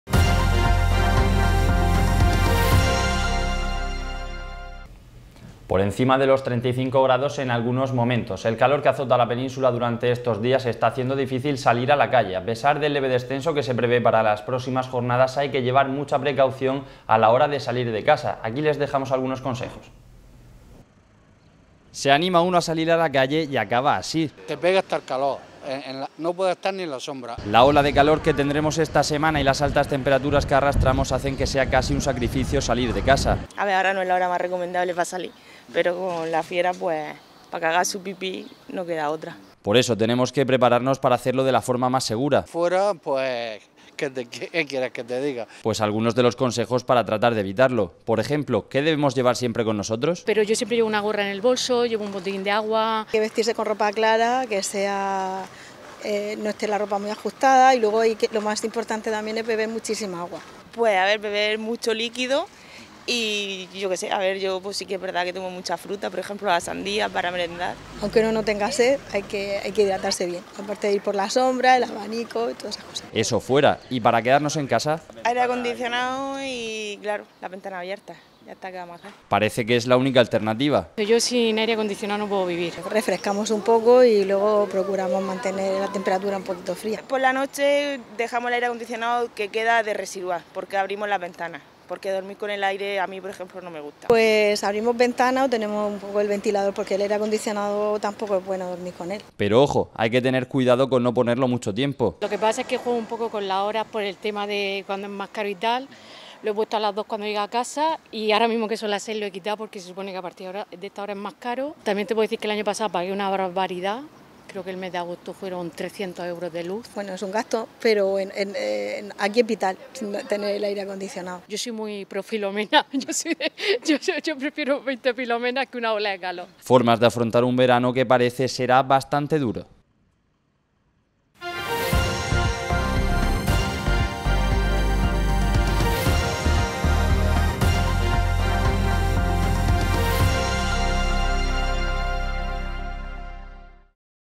¿Cómo combatir las altas temperaturas?
Por encima de los 35 grados en algunos momentos del día. El calor que azota la península durante estos días está haciendo difícil salir a la calle. A pesar del leve descenso que se prevé para las próximas jornadas hay que llevar mucha precaución a la hora de salir de casa. Aquí les dejamos algunos consejos.